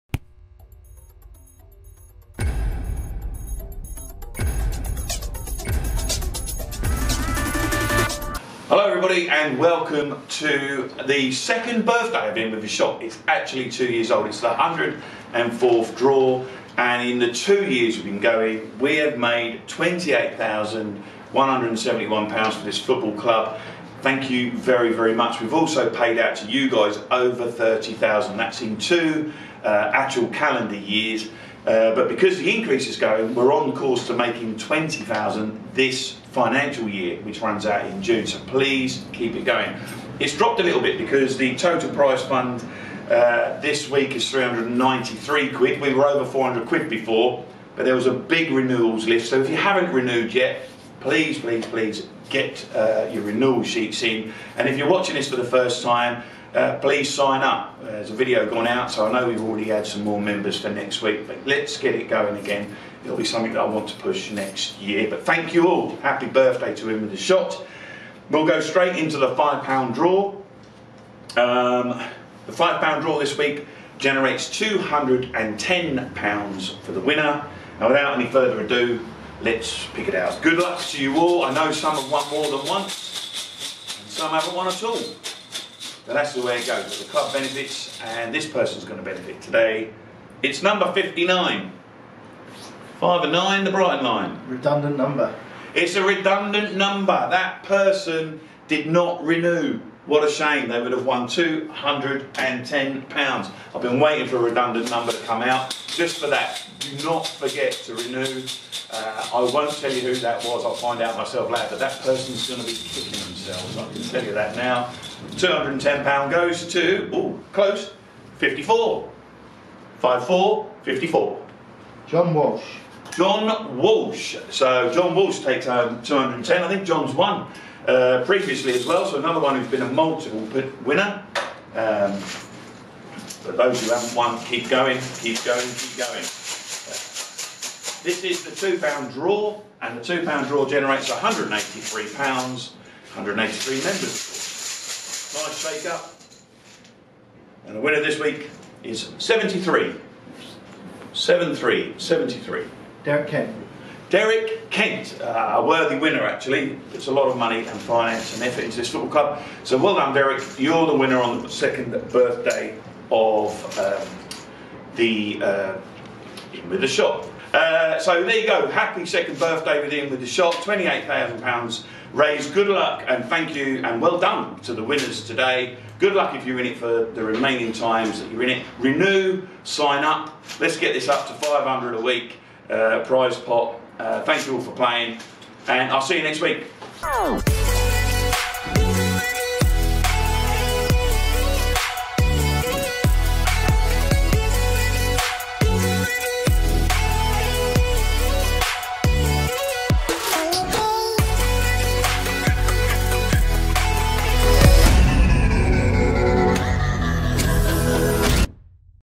IN WITH A SHOT: Week 104 Winners! (21st January 2022)
The winners of Week 104 Winners (21st January 2022) have been announced by Commercial Manager Mark Butler!